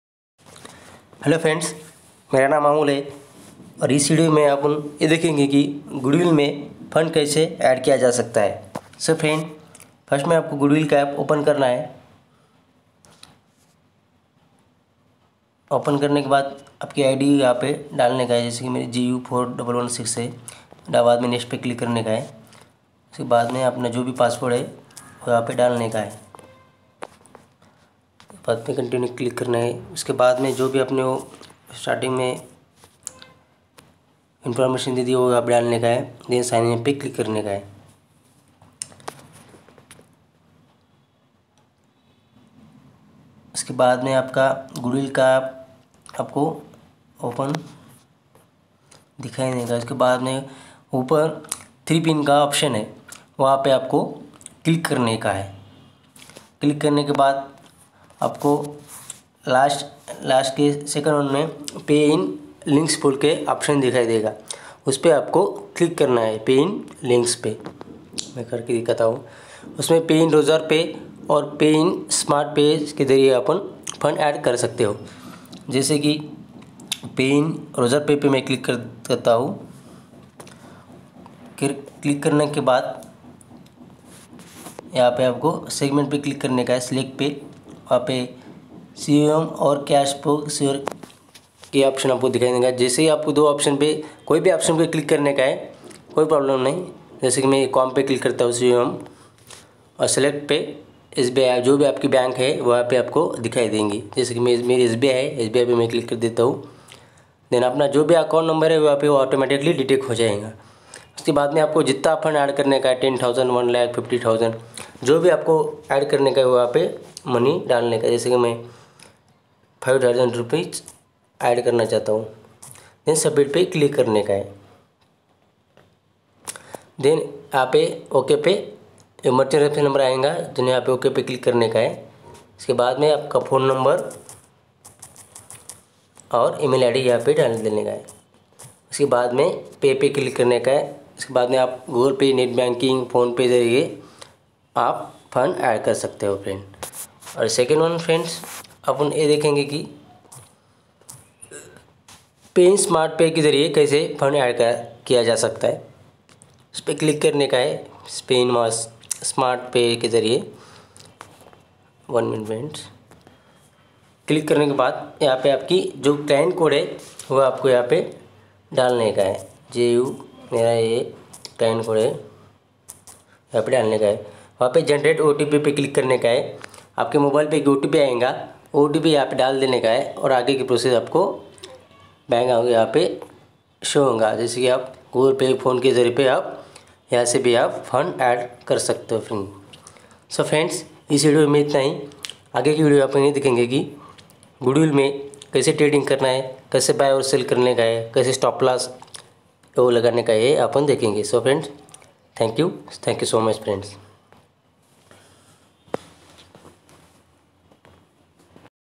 How to Pay in Funds - GOODWILL GIGA- IN HINDHI
BEST PAYIN APP GOODWILL GIGA. HOW TO PAY IN FUND AND FUND TRANSFER CLEARELY SHOWN  IN THIS APP